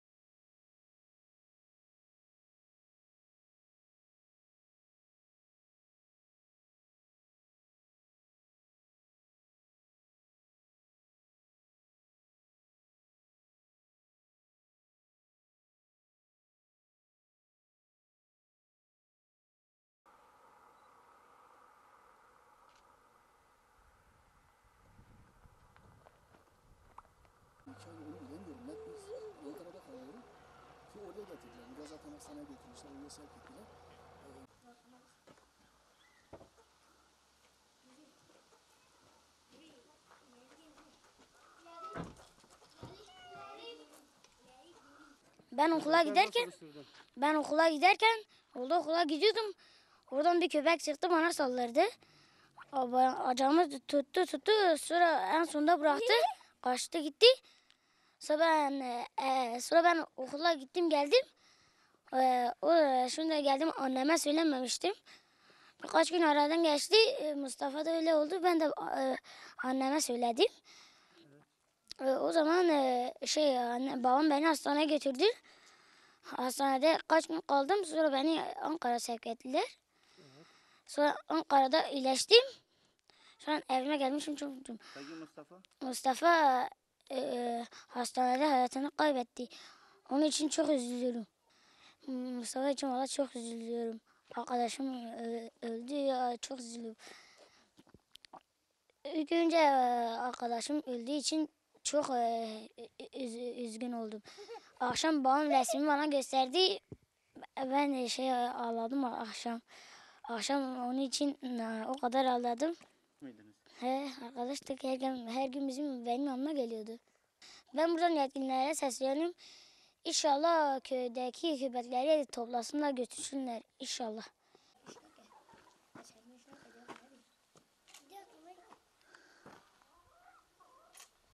Bitlis'te kuduzdan hayatını kaybeden Mustafa'nın arkadaşları konuştu: Çok üzüldüm
Bitlis'in Adilcevaz ilçesinin Göldüzü köyünde, sokak köpeklerinin ısırması sonucu Hacettepe Üniversitesi Çocuk Hastanesi'ne sevk edilen ve yapılan tetkiklerde kuduz olmadığı anlaşılınca taburcu edilen 4'üncü sınıf öğrencisi Polat Ergün (10), yaşadıklarını anlattı. Polat, köpekler kendisini ısırdıktan 3 gün sonra olayı annesine anlattığını ve hayatını kaybeden arkadaşı Mustafa Erçetin'e çok üzüldüğünü söyledi.

Saldırı sonrası ilk olarak Adilcevaz Onkoloji Hastanesi'nde müdahalesi yapılan çocuklar, 21 Ekim'de kuduz şüphesi ile Ankara Hacettepe Üniversitesi İhsan Doğramacı Çocuk Hastanesi'ne sevk edildi. Tedaviye alınan Mustafa Erçetin'e kuduz tanısı konulurken, Polat Ergün'ün ise kuduz olmadığı anlaşıldı.

'KÖPEKLERİN SALDIRDIĞINI ANNEME SÖYLEMEDİM'

Polat Ergün, o anları anlatırken, "Köpek ayağımı ısırdıktan sonra bıraktı, ben de okula gittim, akşam da eve geldim. Köpeklerin bana saldırdığını anneme söylemedim. Aradan birkaç gün geçtikten sonra olayı anneme anlattım sonra babam beni hastaneye götürdü. Adilcevaz Onkoloji Hastanesi'nde birkaç gün tedavi gördükten sonra kuduz şüphesiyle beni Hacettepe Üniversitesi Hastanesi'ne sevk ettiler. Yapılan tedavinin ardından iyileşip taburcu oldum. Benim arkadaşım hastanede hayatını kaybetti. Onun ölümüne çok üzüldüm. Babam onun fotoğrafını bana gösterince ben onun için çok ağladım. Biz arkadaştık. Her gün benim yanıma geliyordu. Mustafa 6'ncı sınıfta, ben de 4'üncü sınıfta okuyorum. Bu olay 1 ay önce oldu. Yetkililere sesleniyorum. Köydeki köpekleri toplayıp götürsünler. Bir daha böyle olaylar olmasın" dedi.